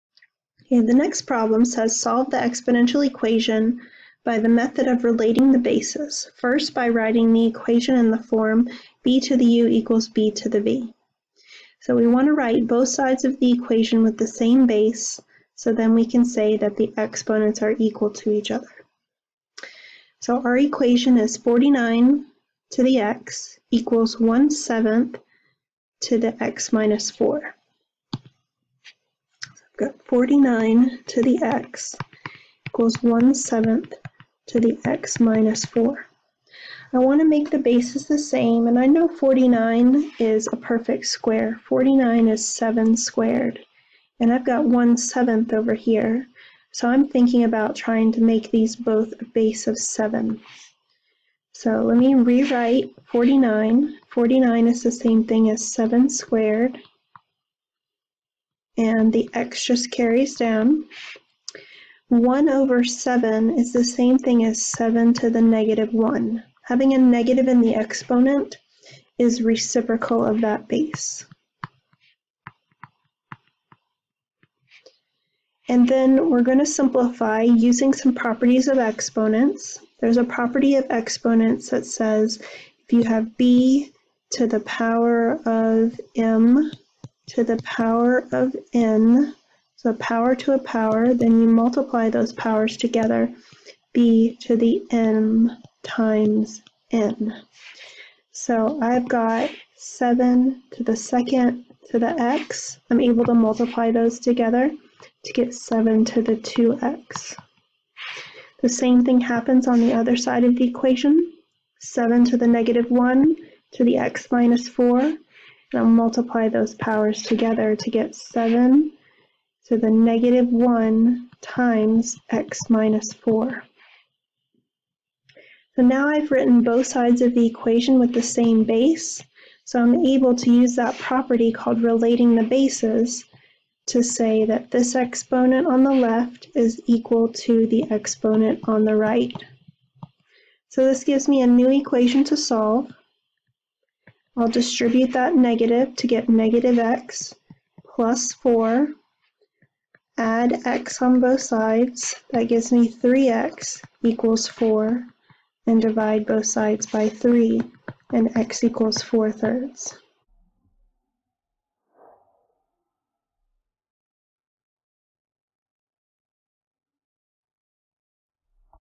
Solve an Exponential Equation by Relating the Bases
Solve the exponential equation by relating the bases otherwise know as the one to one property.  You will also use one of the properties of exponents called the power rule.

Website Posts on Solving Exponential Equations by Making the Bases the Same